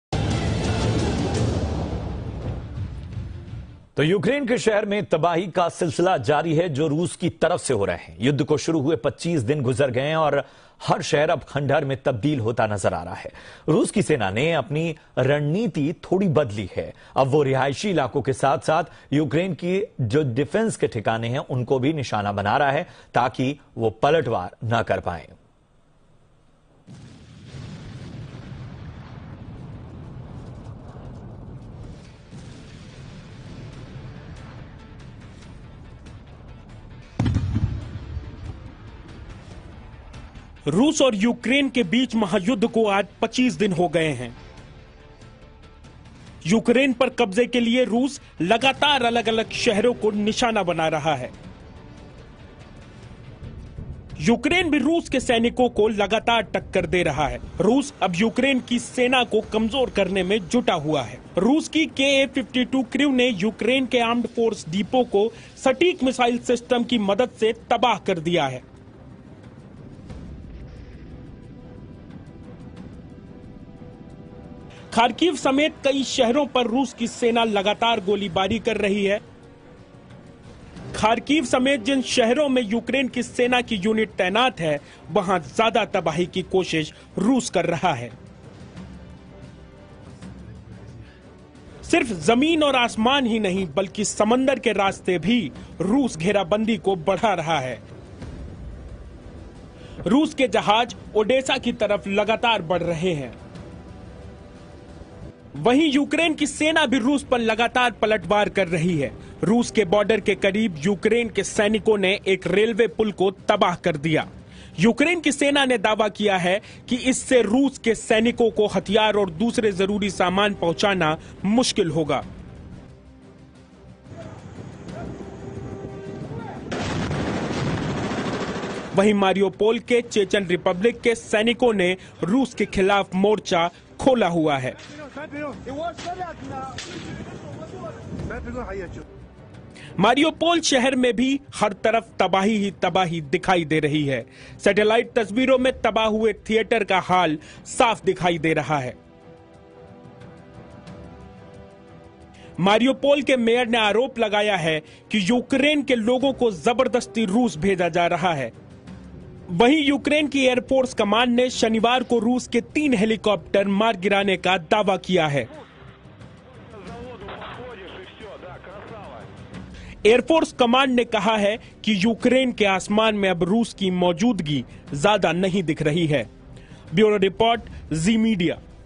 Ukraine city 'wiped off from Earth's face' Watch this video to know 25th day updates
🔸यूक्रेन के शहर में तबाही का सिलसिला जारी

🔸रूस-युक्रेन युद्ध को 25 दिन पूरे

🔸खंडहर में तब्दील हूआ देश का हर शहर

देखिए ये खास रिपोर्ट